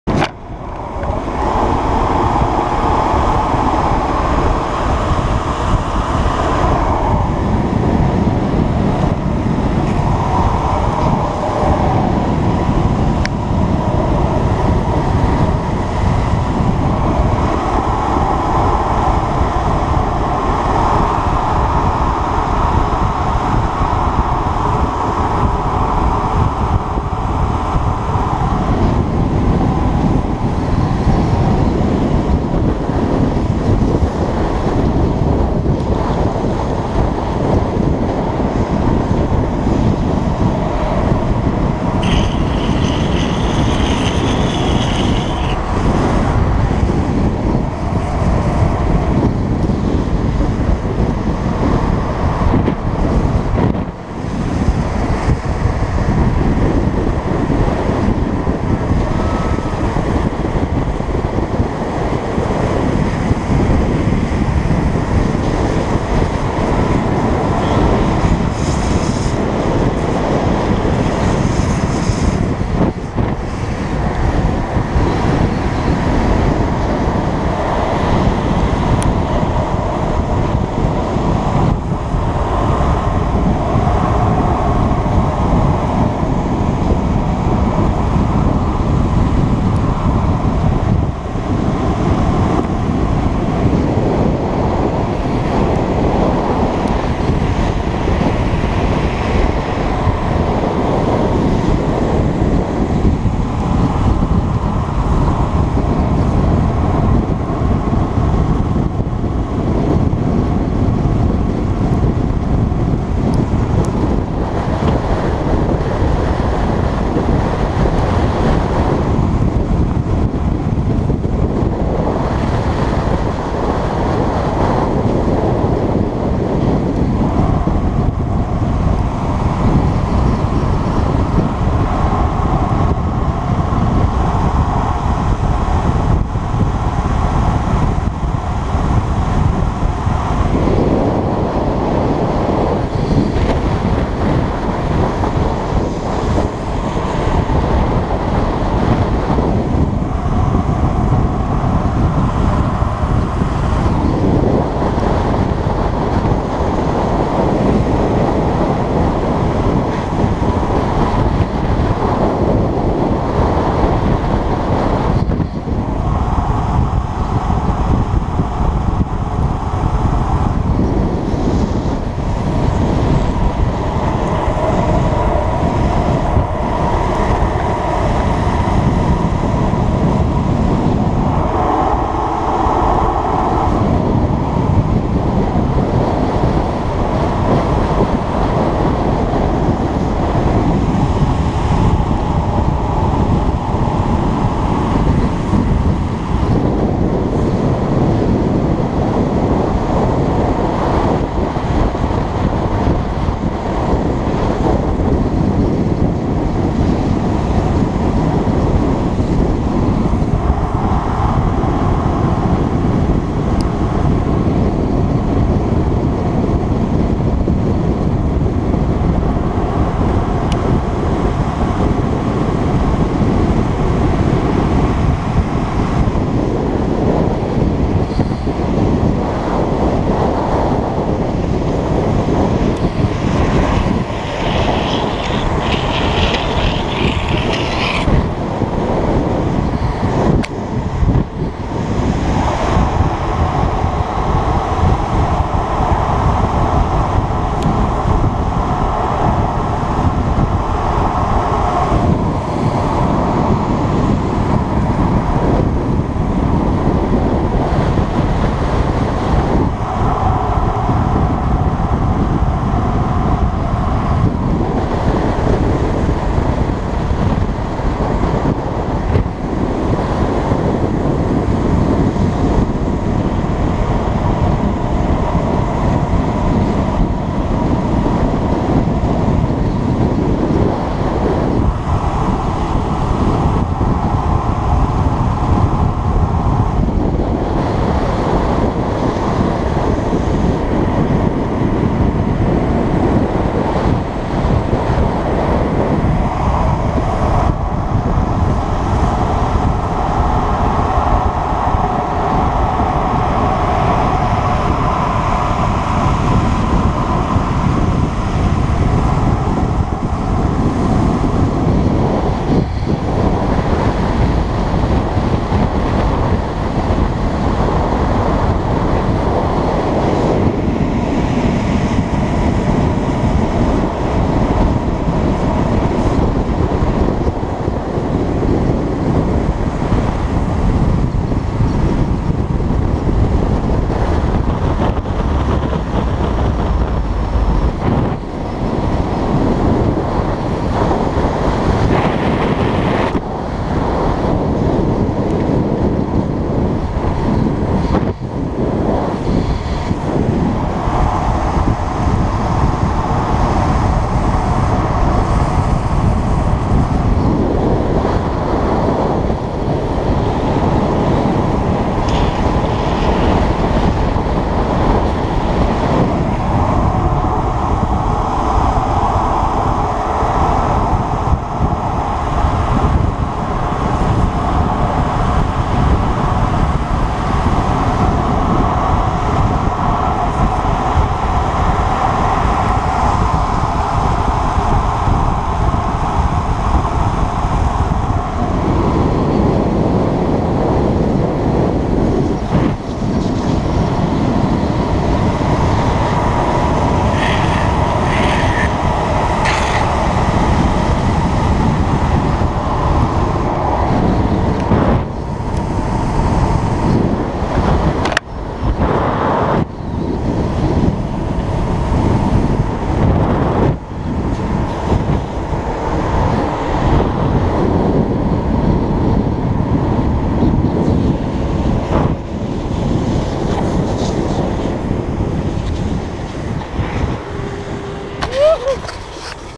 Sweet Potato
Members of Team 2old4roadrash, Team Paproski and Team Mile High Border Patrol found a new spot to dawn patrol, eat breakfast or lunch while commuting to and from Maryhill Loops Road. Friendly locals and not far from the Interstate and a decent IHOP.